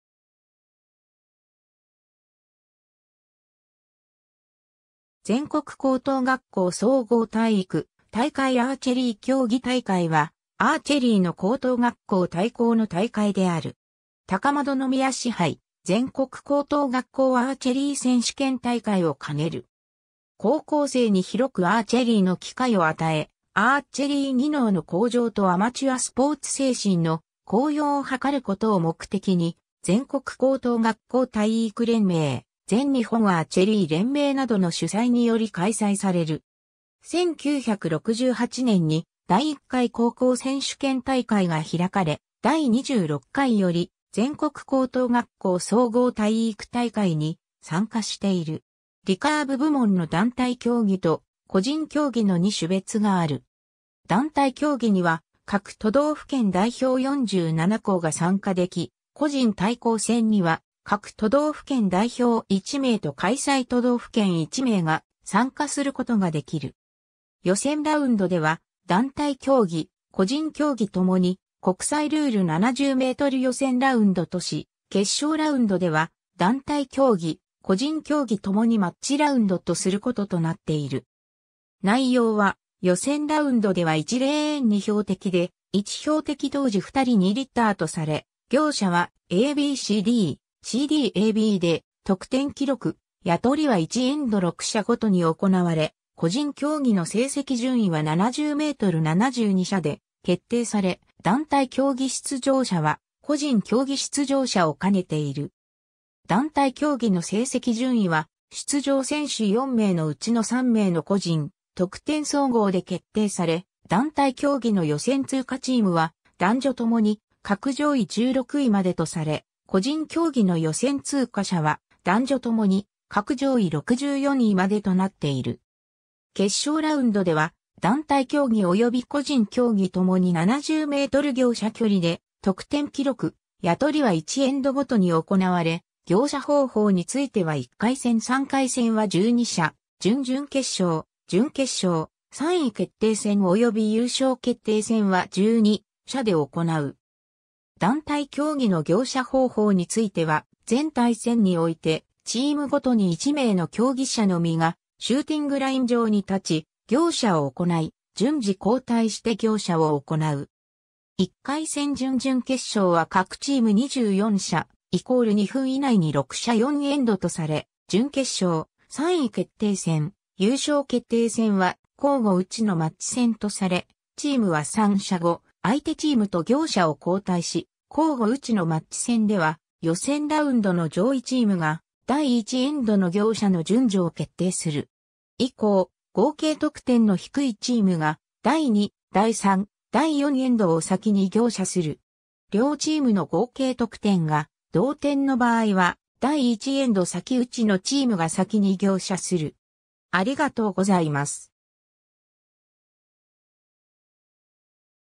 全国高等学校総合体育大会アーチェリー競技大会
全国高等学校総合体育大会アーチェリー競技大会（ぜんこくこうとうがっこうそうごうたいいくたいかいアーチェリーきょうぎたいかい）は、アーチェリーの高等学校対抗の大会である。
高円宮賜牌全国高等学校アーチェリー選手権大会を兼ねる。
高校生に広くアーチェリーの機会を与え、アーチェリー技能の向上とアマチュアスポーツ精神の高揚をはかることを目的に、全国高等学校体育連盟、全日本アーチェリー連盟などの主催により開催される。
1968年に第1回高校選手権大会が開かれ、第26回（1993年）より全国高等学校総合体育大会に参加している。
 リカーブ部門の団体競技と個人競技の2種別がある。
団体競技には、各都道府県代表47校が参加でき、個人対抗戦には、各都道府県代表1名と開催都道府県1名が参加することができる。
 予選ラウンドでは団体競技・個人競技ともに国際ルール70m予選ラウンドとし、決勝ラウンドでは団体競技・個人競技ともにマッチラウンドとすることとなっている。
内容は予選ラウンドでは1レーン2標的で、1標的同時2人2立（AB・CD）とされ、行射はAB・CD/CD・ABで得点記録・矢取りは1エンド6射ごとに行われ、個人競技の成績順位は70m72射で決定され、団体競技出場者は個人競技出場者を兼ねている。
団体競技の成績順位は出場選手4名の内の3名（同一校）の個人得点総合で決定され、団体競技の予選通過チームは、男女ともに各上位16位までとされ、個人競技の予選通過者は、男女ともに各上位64位までとなっている。
 決勝ラウンドでは団体競技及び個人競技ともに70m行射距離で得点記録・矢取りは1エンドごとに行われ、行射方法については1回戦～3回戦は12射（4分6射を2エンド）、準々決勝・準決勝・3位決定戦及び優勝決定戦は12射（1射30秒以内に3射を4エンド）で行う。
団体競技の行射方法については、全対戦においてチームごとに1名の競技者の身がシューティングライン上に立ち、行射を行い、順次後退して行射を行う。
1回戦～準々決勝は各チーム24射=2分以内に6射（3名×2射）×4エンドとされ、準決勝・3位決定戦・優勝決定戦は、交互射ちのマッチ戦とされ、チームは3射（1射×3名）後、相手チームと行射を交代し、交互射ちのマッチ戦では、予選ラウンドの上位チームが、第1エンドの行射の順序を決定する。
以降、合計得点の低いチームが第2、第3、第4エンドを先に行射する。
両チームの合計得点が同点の場合は第1エンド先射ちのチームが先に行射する。